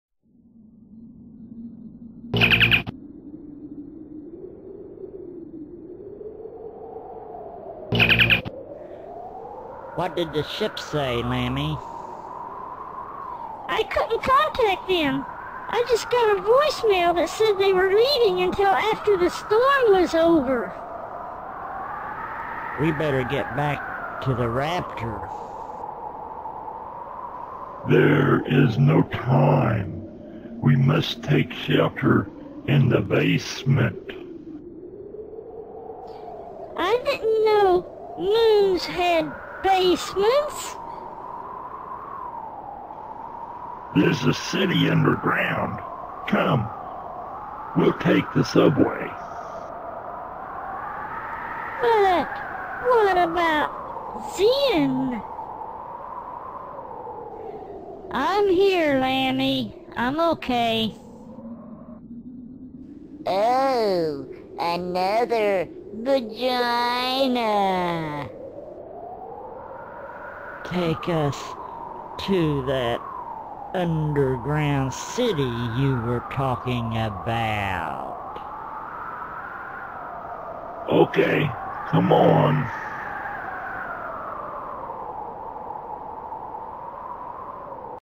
Moon Basement
The girls must find shelter from the solar storm.